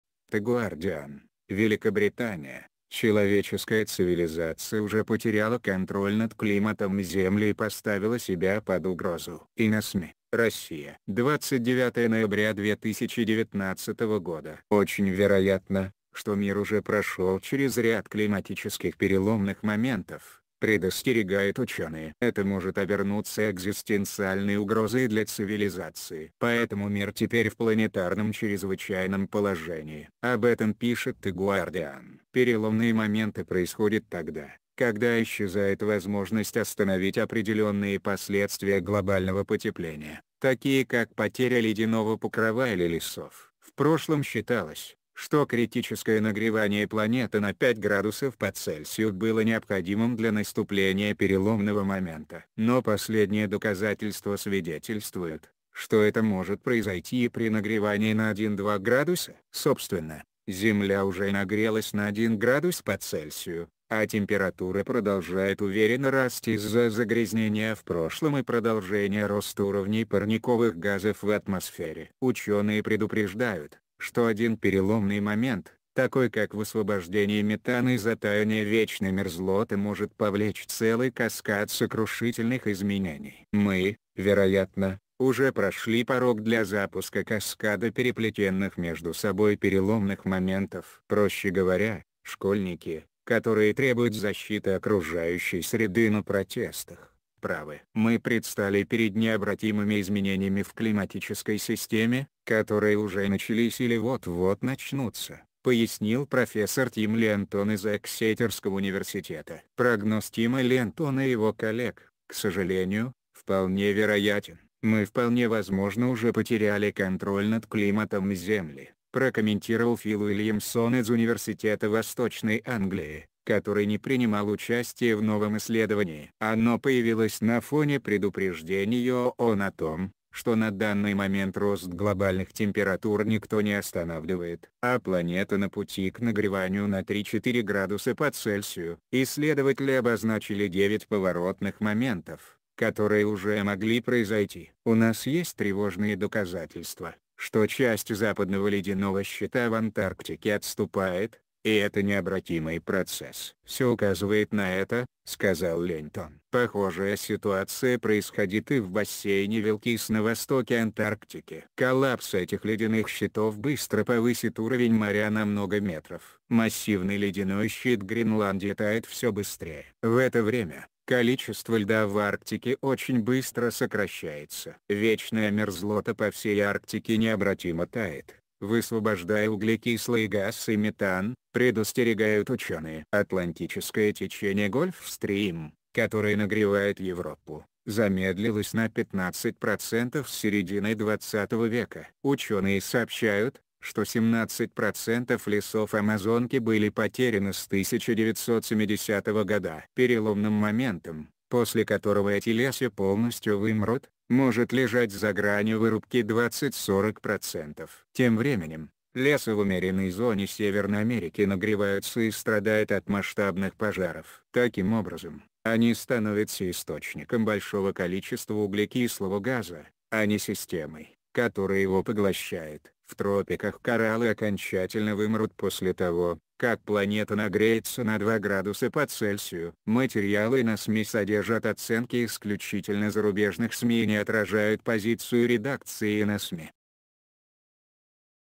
The Guardian (Великобритания): человеческая цивилизация уже потеряла контроль над климатом Земли и п
The Guardian (Великобритания): человеческая цивилизация уже потеряла контроль над климатом Земли и поставила себя под угрозу.
ИноСМИ, Россия.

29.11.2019

Очень вероятно, что мир уже прошел через ряд климатических переломных моментов, — предостерегают ученые. Это может обернуться «экзистенциальной угрозой для цивилизации». Поэтому мир теперь в «планетарном чрезвычайном положении». Об этом пишет The Guardian .  Переломные моменты происходят тогда, когда исчезает возможность остановить определенные последствия глобального потепления, такие как потеря ледяного покрова или лесов. В прошлом считалось, что критическое нагревание планеты на 5 градусов по Цельсию было необходимым для наступления переломного момента. Но последние доказательства свидетельствуют, что это может произойти и при нагревании на один-два градуса.  Собственно, Земля уже нагрелась на один градус по Цельсию, а температура продолжает уверенно расти из-за загрязнения в прошлом и продолжения роста уровней парниковых газов в атмосфере. Ученые предупреждают, что один переломный момент, такой как высвобождение метана из-за таяния вечной мерзлоты может повлечь целый каскад сокрушительных изменений.  «Мы, вероятно, уже прошли порог для запуска каскада переплетенных между собой переломных моментов. Проще говоря, школьники, которые требуют защиты окружающей среды на протестах, правы. Мы предстали перед необратимыми изменениями в климатической системе, которые уже начались или вот-вот начнутся», — пояснил профессор Тим Лентон из Эксетерского университета. «Прогноз Тима Лентона и его коллег, к сожалению, вполне вероятен. Мы вполне возможно уже потеряли контроль над климатом З...